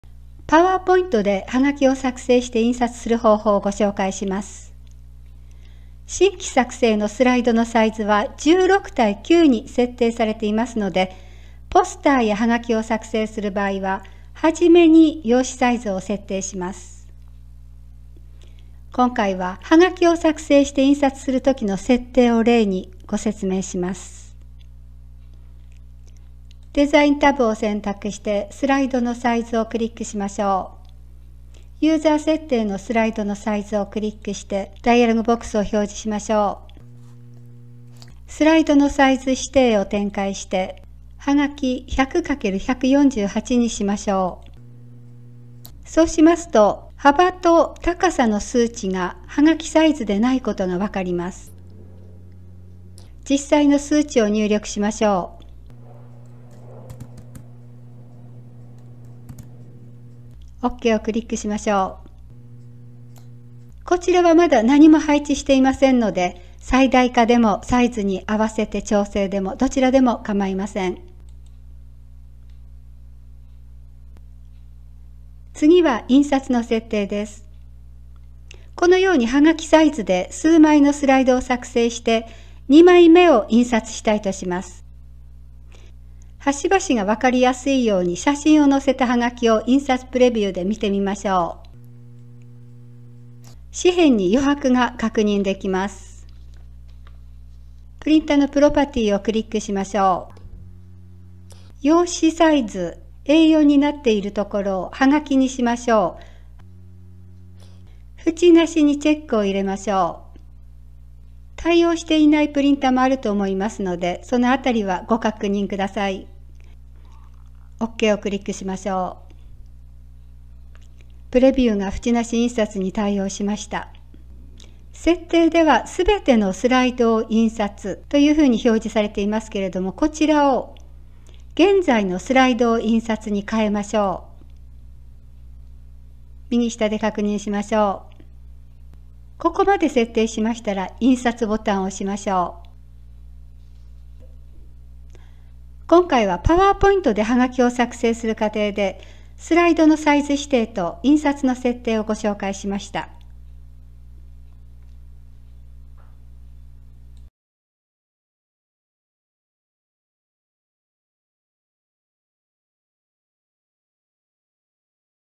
【パワーポイント_はがき作成+印刷】PowerPoint_スライドのサイズ指定
新規作成のスライドのサイズは16：9に設定されています。
ポスターやハガキを作成して印刷する場合は、初めに用紙サイズを設定します。
合わせて印刷の手順もご紹介します♪

「山歩き」と「パソコンLife」を中心とした 日々の活動の動画です。
よろしければ下記のホームページもご覧ください。
パソコン教室あっぷ… 👇👇 作品のダウンロードはこちらから